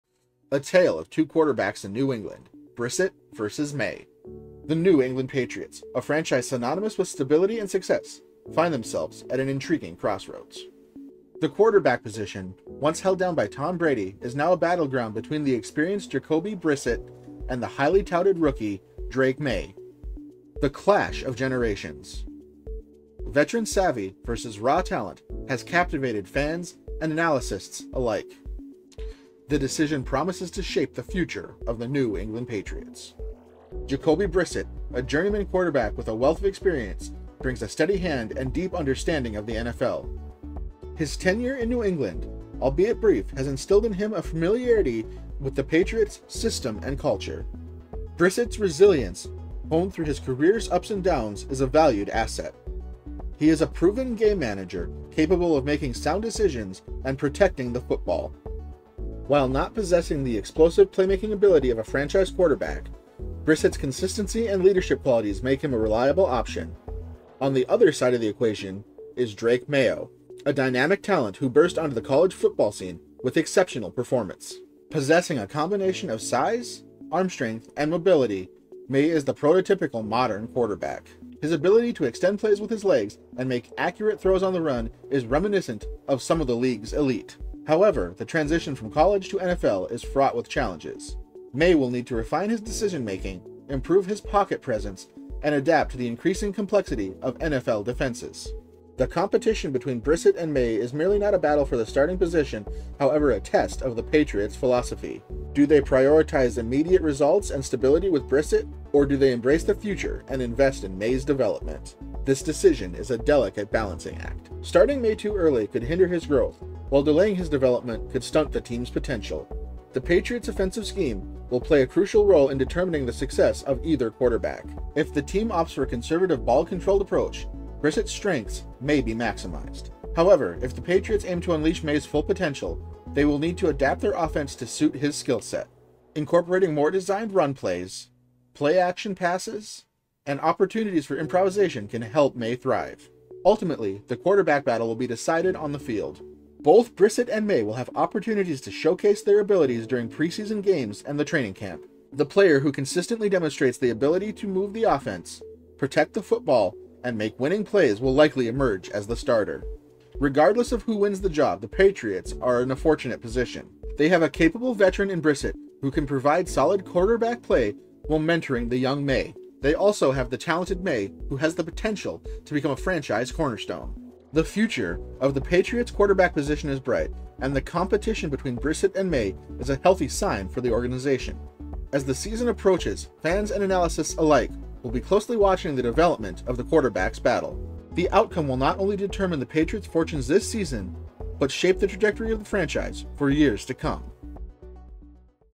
The quarterback battle in New England is getting complicated ....
The quarterback battle in New England between Drake Maye and Jacoby Brissett is shaping up to be an intriguing storyline for the upcoming NFL season. Drake Maye, a promising rookie fresh out of college, brings a youthful dynamism and a strong arm to the Patriots' roster. His college career was marked by impressive statistics and leadership qualities that have translated well into the professional arena. On the other hand, Jacoby Brissett is a seasoned veteran who is already familiar with the Patriots' system, having begun his career in New England. Brissett's experience and calm demeanor under pressure make him a reliable choice, especially in high-stakes situations. The decision between Maye's potential and Brissett's experience will ultimately boil down to the team's strategic priorities and long-term vision. Coach Bill Belichick's decision-making will be under the spotlight as fans eagerly await which quarterback will lead the Patriots this season.